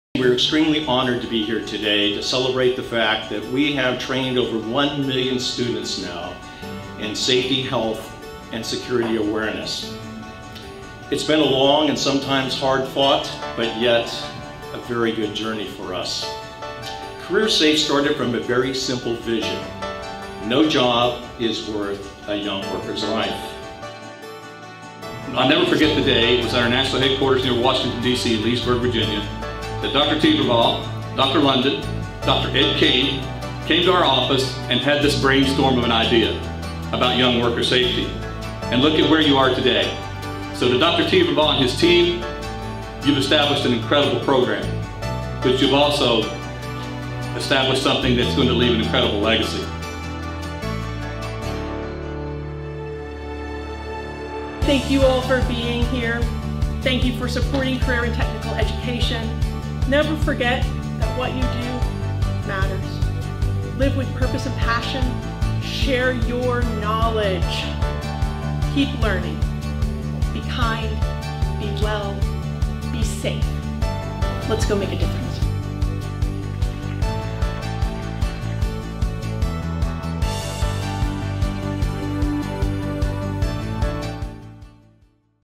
CareerSafe | 'A Million Safer' Highlight Video
CareerSafe is proud to have reached a major milestone by training over one million students in safety training! Check out this highlight of our 'A Million Safer' event that celebrated this achievement in Austin, Texas.